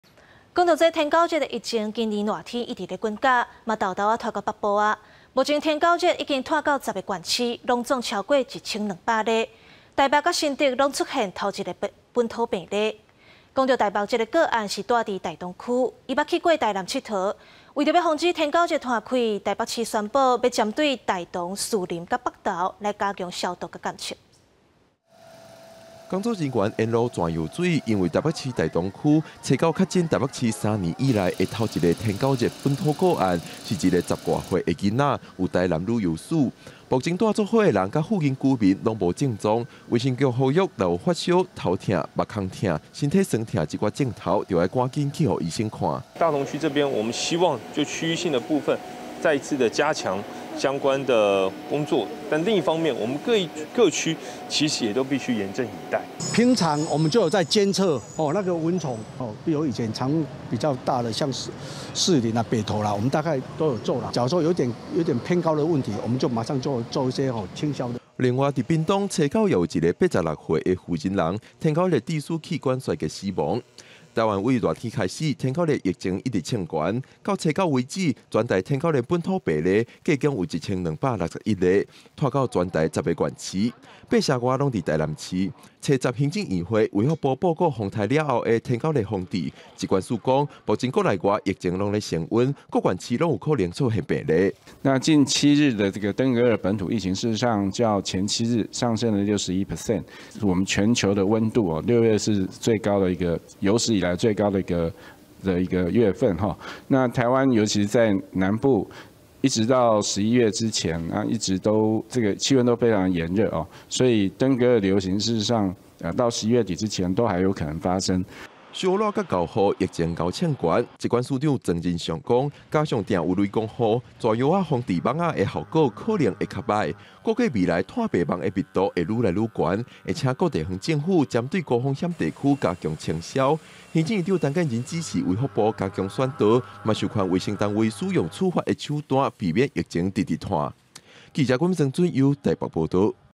臺北大同出現頭例本土天狗熱 捌去臺南𨑨迌 | 公視台語台 | 20230810
台灣天狗熱疫情，對熱天開始就一直衝懸，疾管署也講今年6月較熱，也予國內外疫情gau夯懸。屏東昨昏(9)也有一个80外歲的婦人人，因為天狗熱致使多重器官衰竭死亡；台北市佇仝一天，也出現三年以來頭一例天狗熱本土個案。到初九為止，全台天狗熱本土個案已經有1261例，遍布全台10个縣市。

四節新聞 ：　逐工（每天）09:00 / 12:00 / 15:00（拜一至拜五） / 19:00